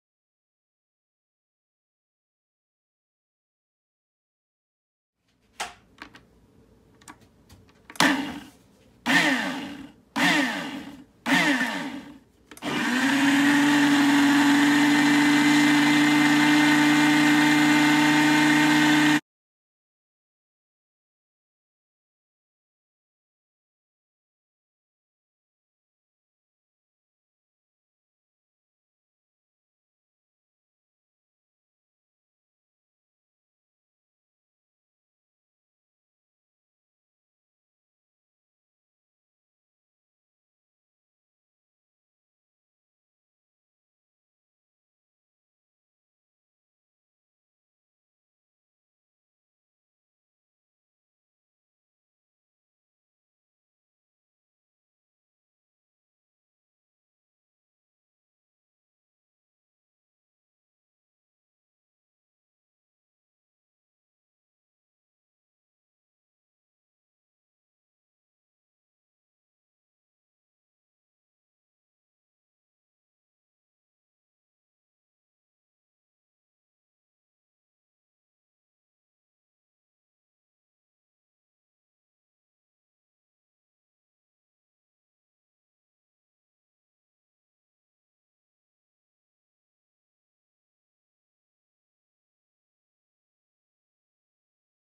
문화창조] 크리스마스 이브에 생긴일🎄사과쥬스 갈다가 믹서기 고장
크리스마스 이브에 생긴일🎄사과쥬스 갈다가 믹서기 고장